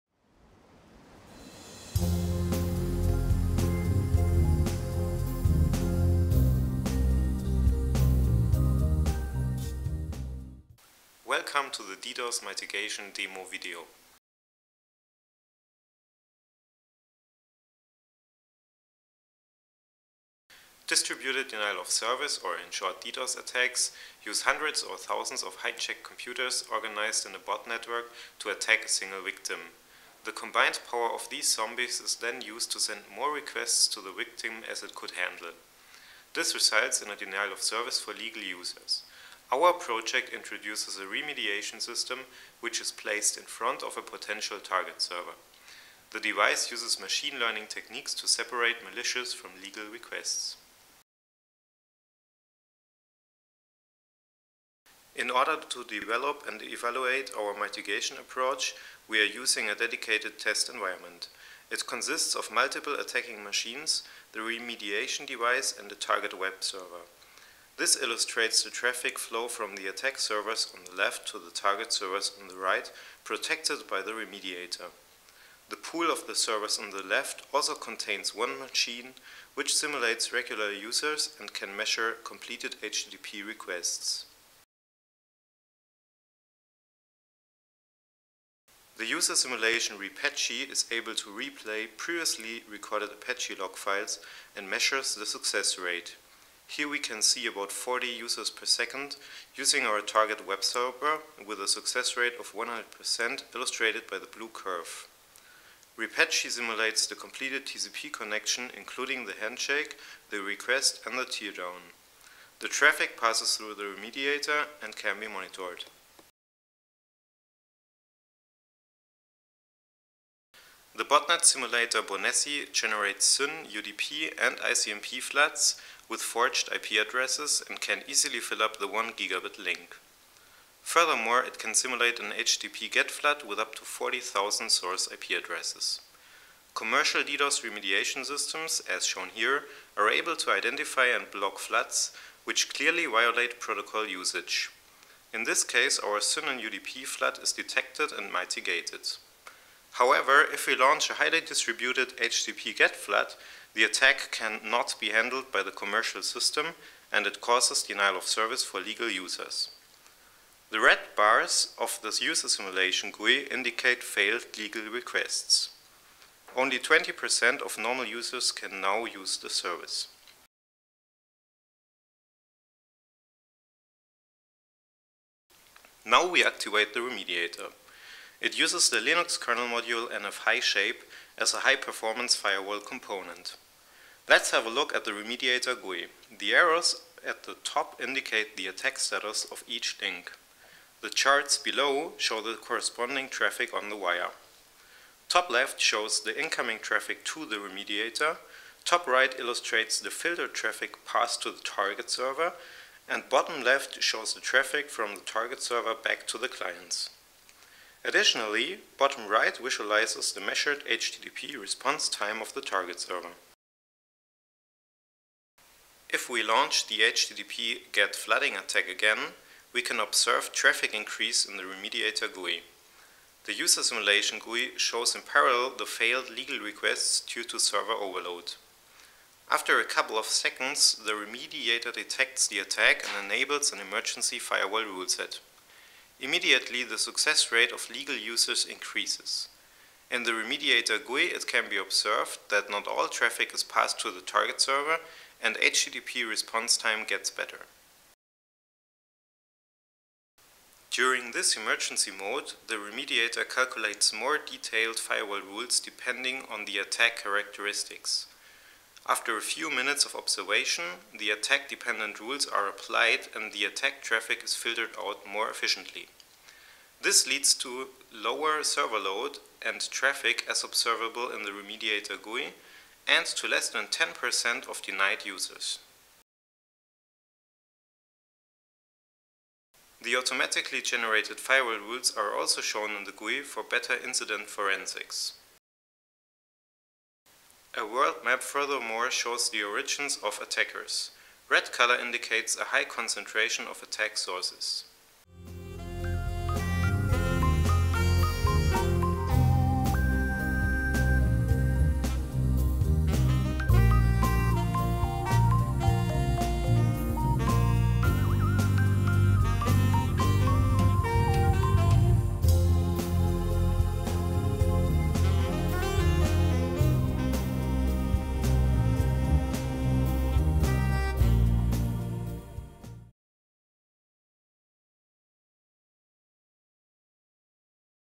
DDos Mitigation by Machine Learning
Mitigating advanced DDoS Attacks is a tough task. This video shows a system, which is able to mitigate even sophisticated HTTP-GET attacks using machine learning techniques. Furthermore, multiple Open Source tools are shown in action: "BoNeSi" the botnet simulator, "repache" a HTTP user simulation and "nf-hishape" a high performance